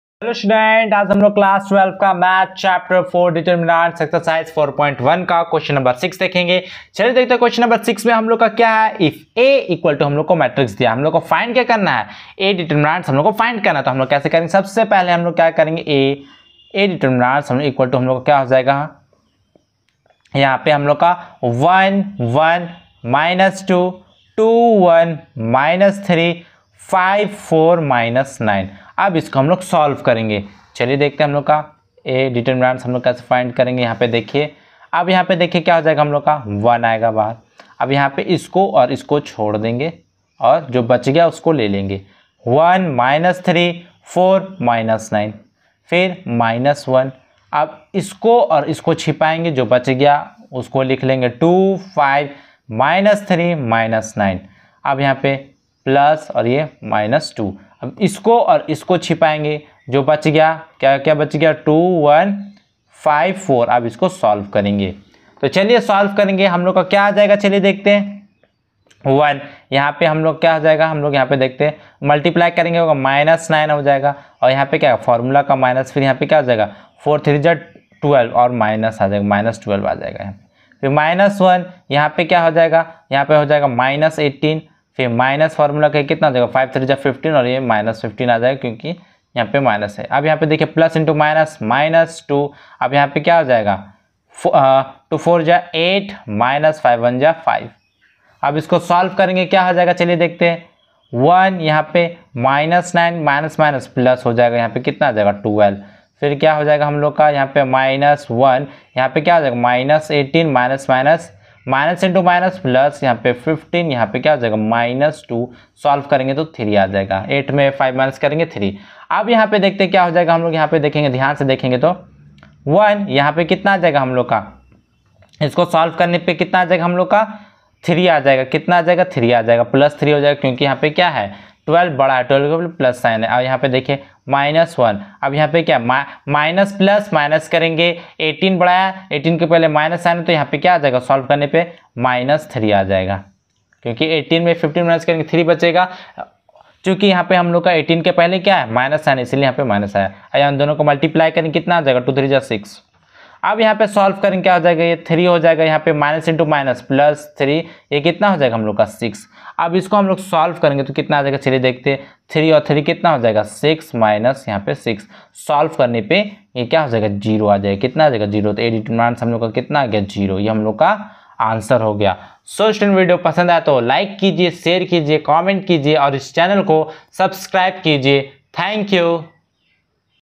Class 12 || Determinants Ex:-4.1 || Question no.6 Answer.
Class 12 || Determinants Ex:-4.1 || Question no.6 If A=[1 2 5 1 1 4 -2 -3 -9] find |A|